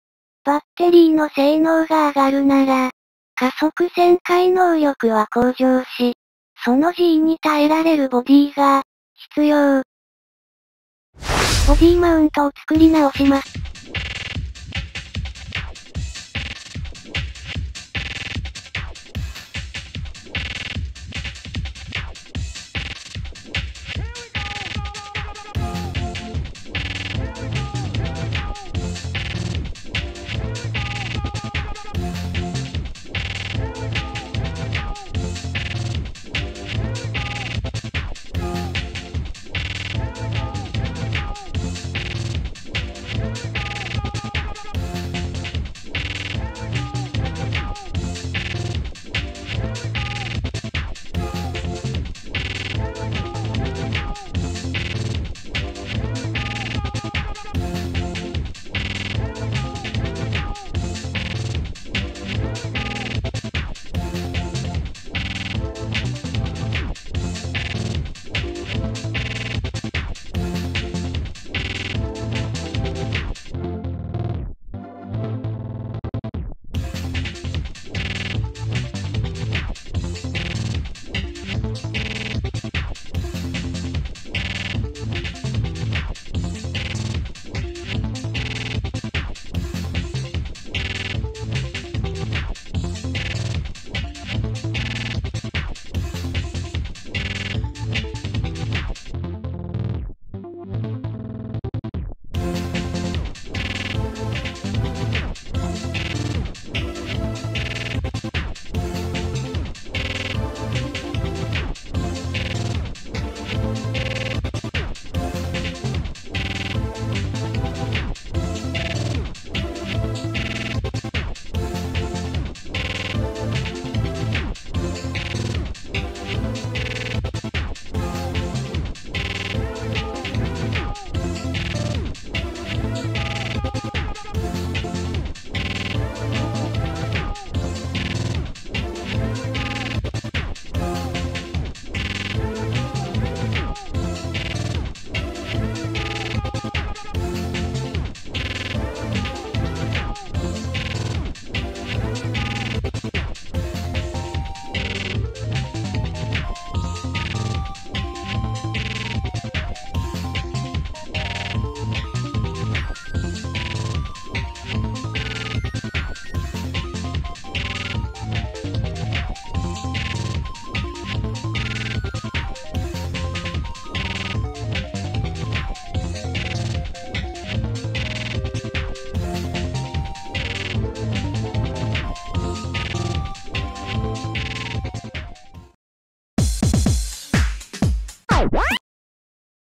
【ラジコン】ボディマウントを作り直した
ボディポストの延長
ボディの穴あけ
マウント作り
など